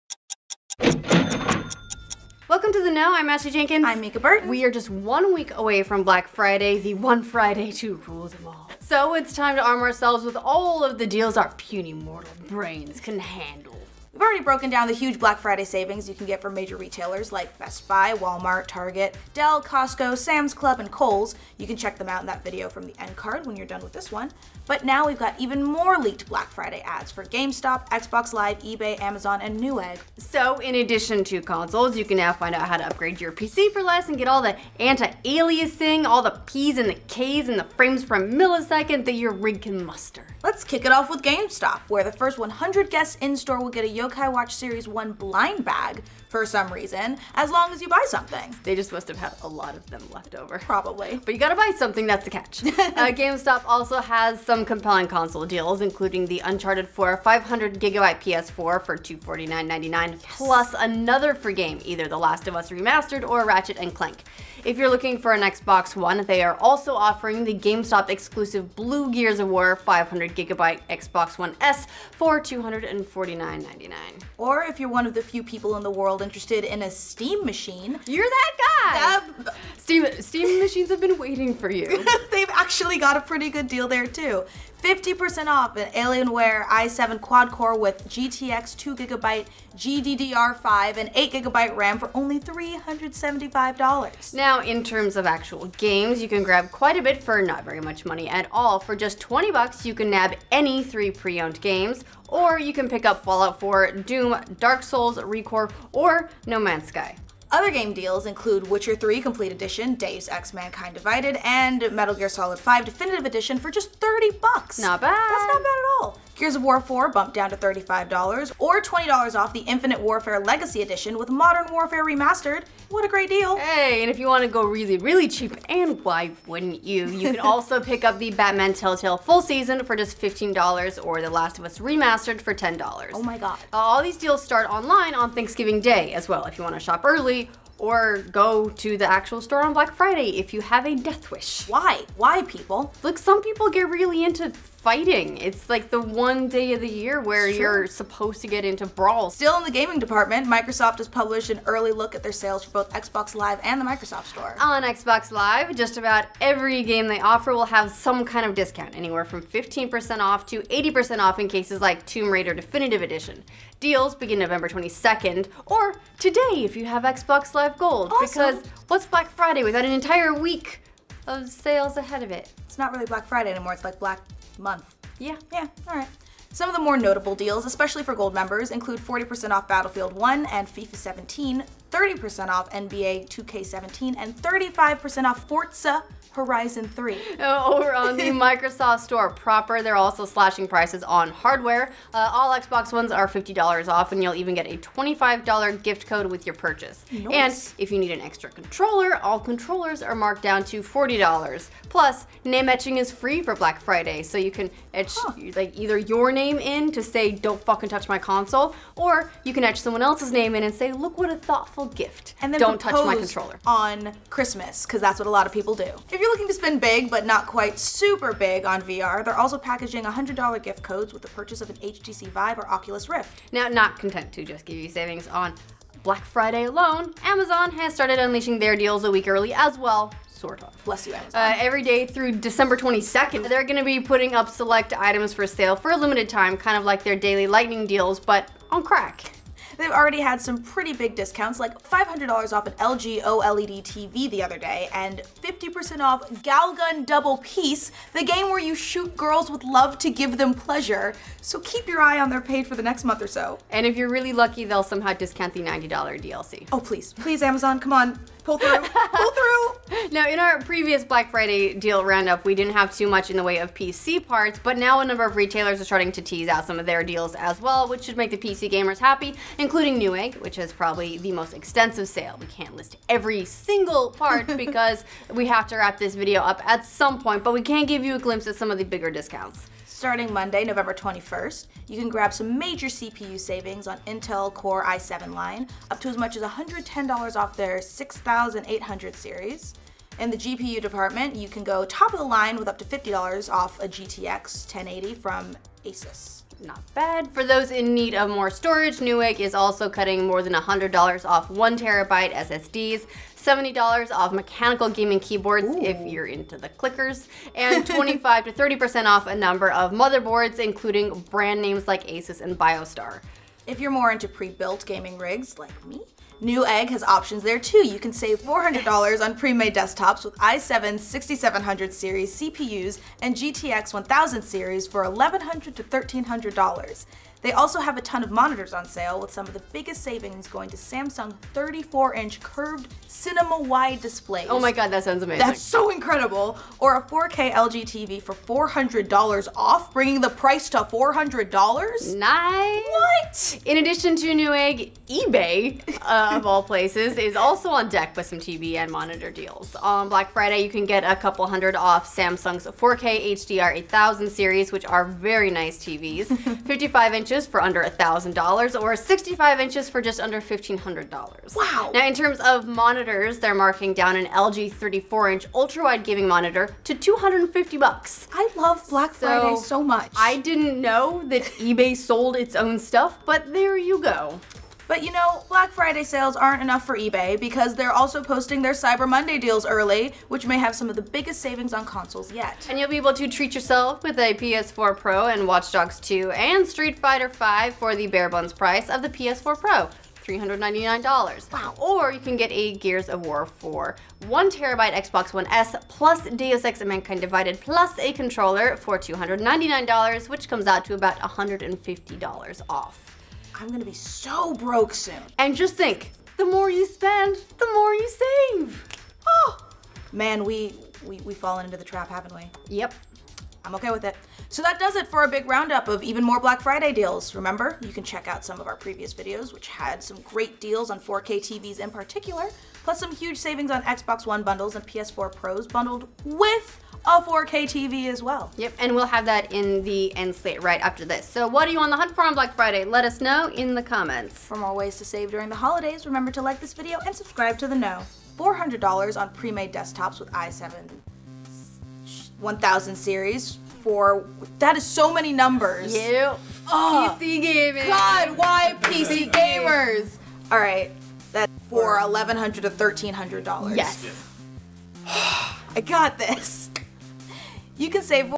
Black Friday BEST PC and CONSOLE GAMING DEALS - The Know Game News
Looking for deals on PC gaming hardware? Games on the cheap? Consoles for a song? Here's a collection of all the latest and greatest Black Friday deals... some of which are actually available starting now!

Hosted By: Ashley Jenkins and Mica Burton